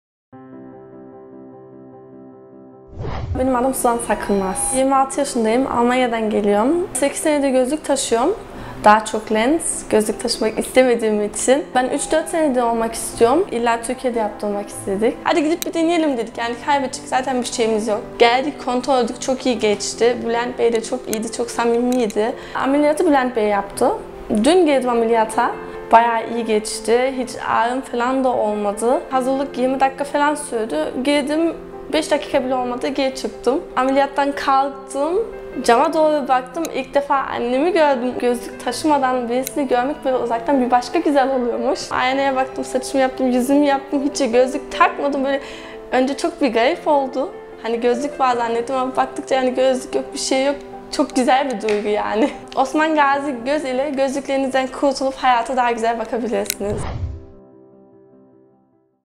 Lazer Ameliyatı Olanlar - Suzan Hanım - SMILE Lazer
-3,75 Miyop ve -1,00 Astigmat sorunuyla Almanya'dan gelerek hastanemize başvuran hastamız Suzan Hanım, Op. Dr. Bülent Köse'nin gerçekleştirdiği SMILE Lazer operasyonu ile gözlüklerine veda etti.

Operasyon öncesinde ve operasyon sonrası kontrol muayenesinde yaptığımız görüşmelerde düşüncelerini bizimle paylaşan hastamız, mutluluğunu da dile getirdi. Kendisine sağlıklı günler dileriz.

Osmangazi Göz SMILE Lazer Danışma ve Randevu Hattı